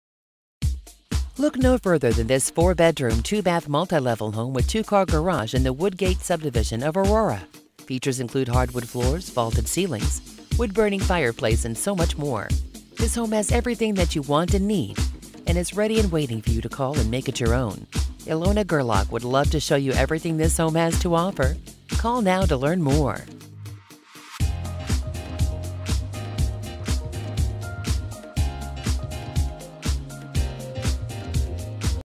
14483 E Wagontrail Place AURORA, CO | $324,900 | coloradohomes.com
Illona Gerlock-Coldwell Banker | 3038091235

Look no further than this 4 bedroom, 3 bathroom Multi-Level home with 2-car garage in the Woodgate subdivision of Aurora. As you enter, the hardwood floors greet your feet and continue throughout the main level. The main and upper levels also boast beautiful vaulted ceilings. The The living room features a wood-burning brick fireplace with wood mantle. The adjacent dining room provides access to the deck and backyard. The kitchen is just off the dining room and is highlighted by the black, Corian-topped island with breakfast bar.